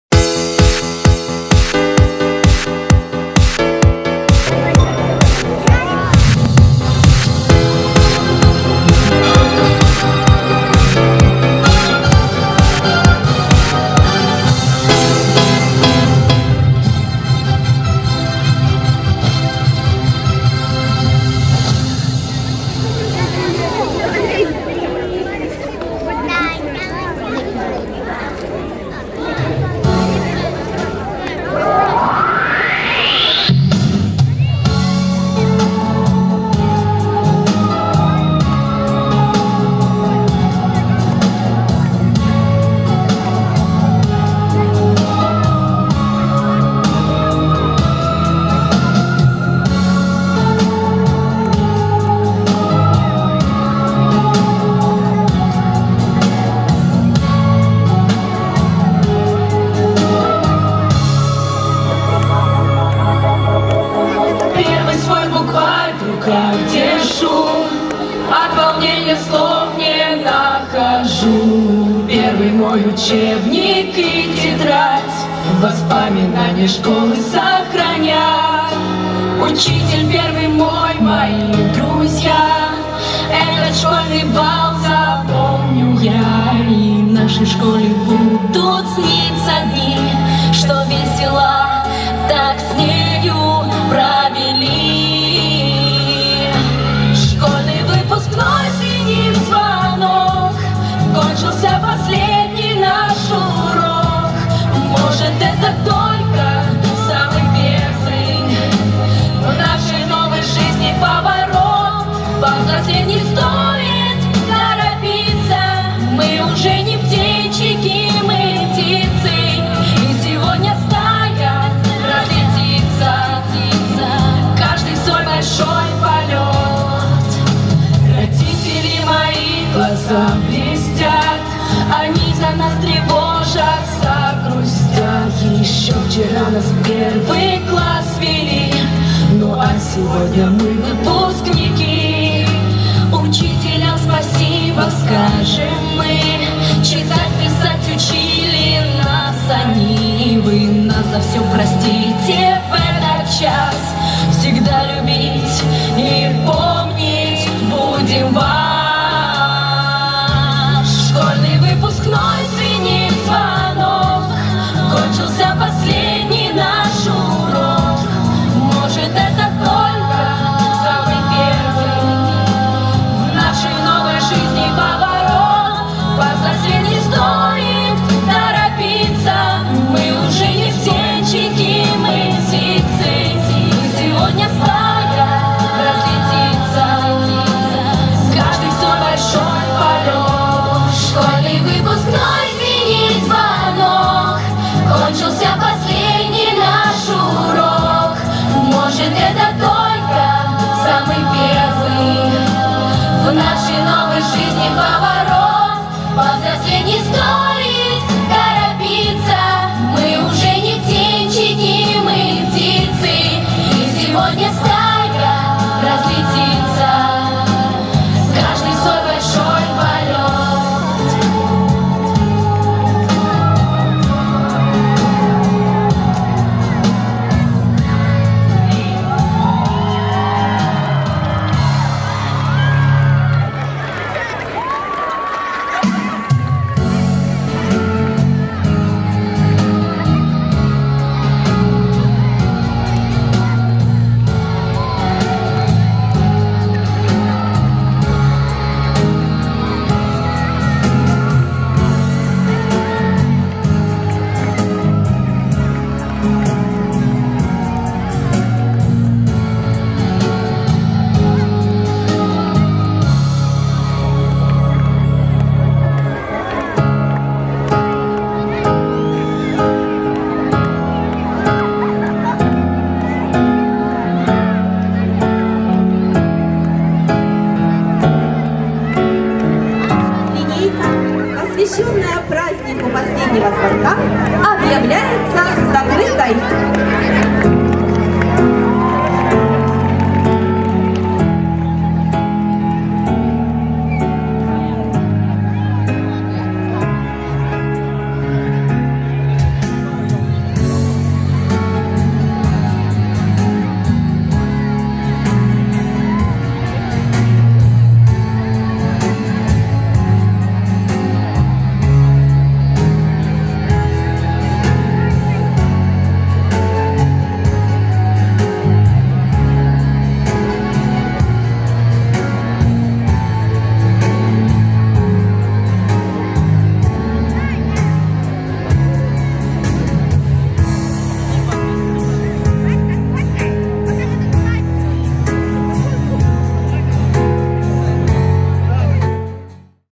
Последний звонок СШ 91 -- трогательная песня выпускников и запуск шаров
Уважаемый …
Если Вам интересны другие материалы нашего производства, то позвольте рекомендовать Вам